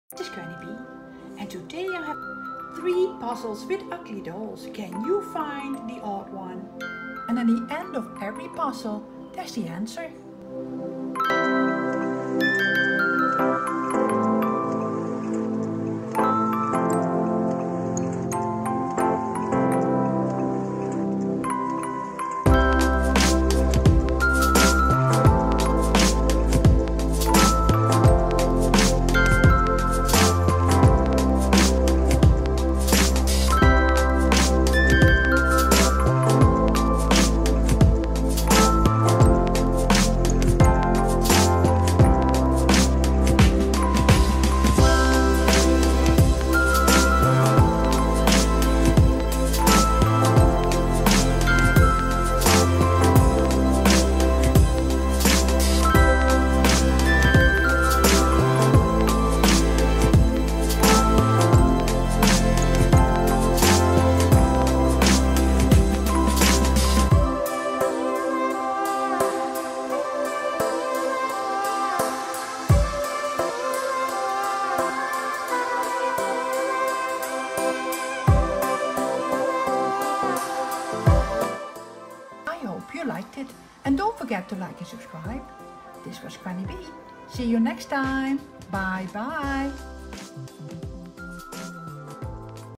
Ugly dolls.. Can you find the odd one ? 3 puzzles with answers. From the new movie Ugly dolls...
Ugly dolls.. Can you find the odd one ?
3 puzzles with answers. From the new movie Ugly dolls...
The new movie Ugly dolls is here and it is so cute..
It is all about accepting and respecting everybody.
Can you find them all?
I am on vacation for 2 weeks...
Please be patient with me...
Granny B. wishes you all a wonderful Summer!